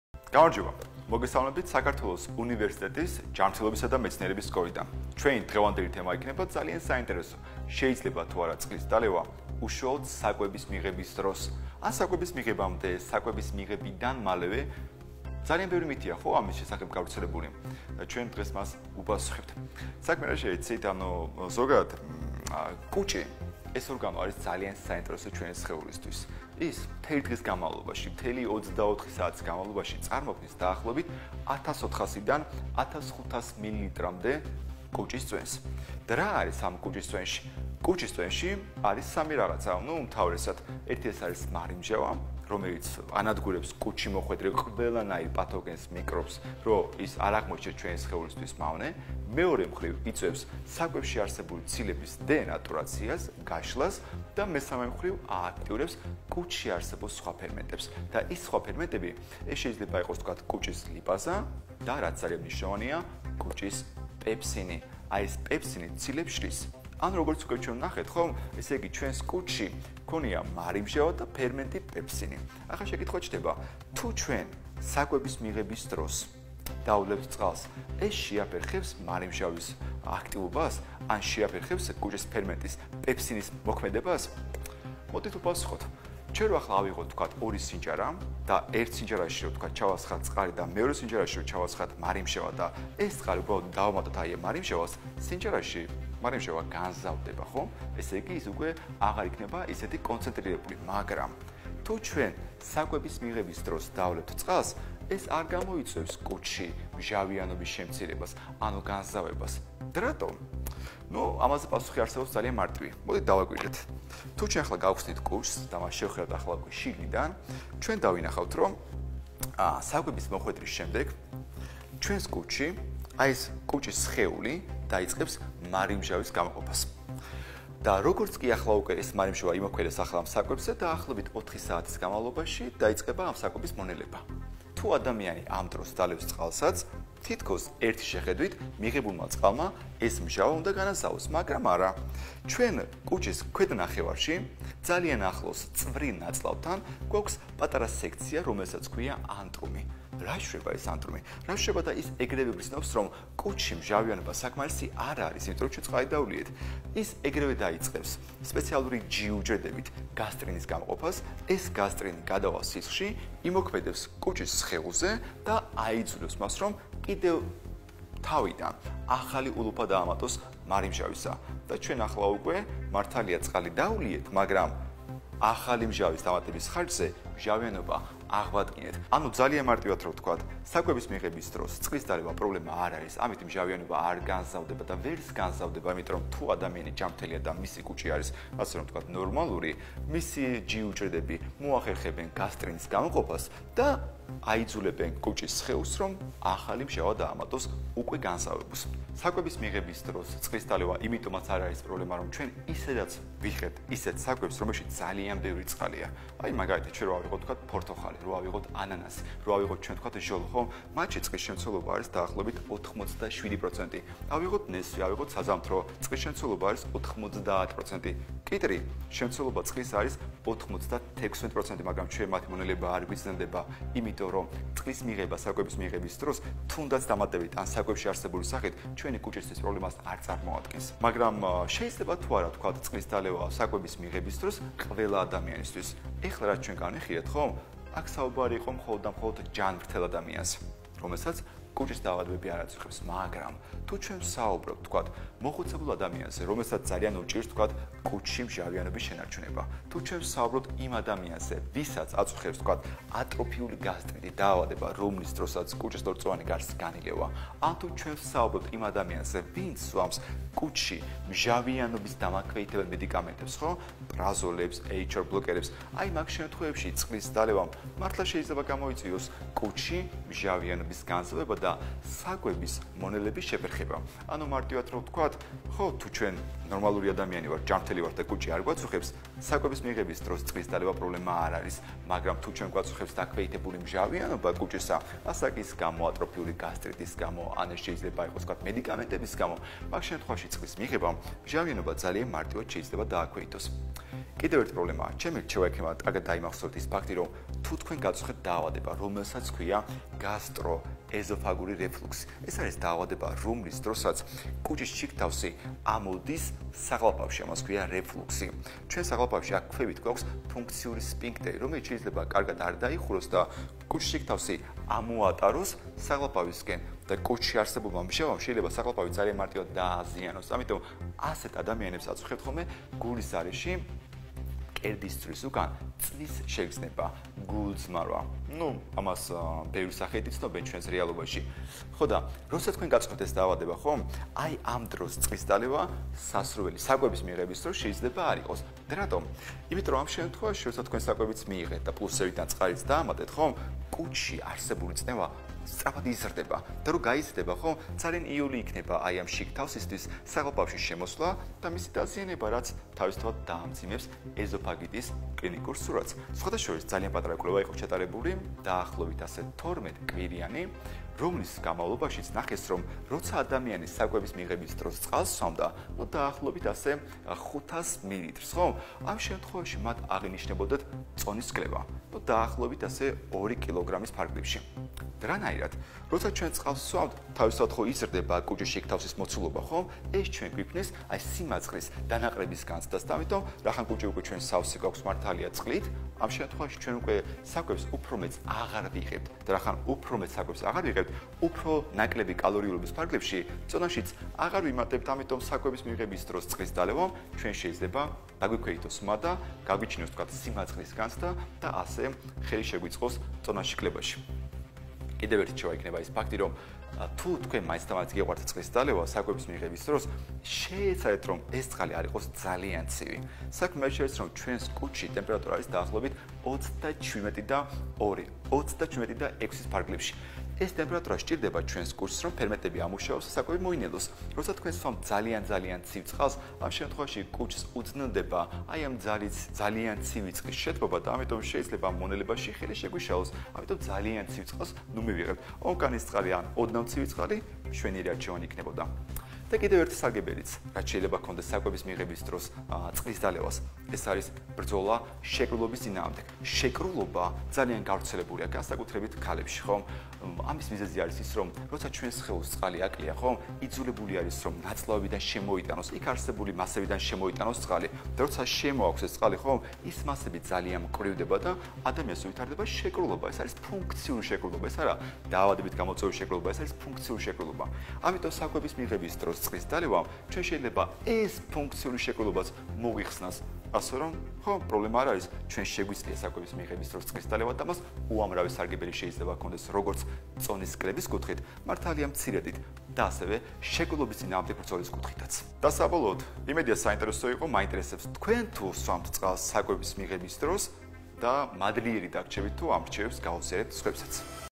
კუჭი, გიორგი ღოღობერიძის რჩევები
შეიძლება თუ არა წყლის დალევა ჭამის დროს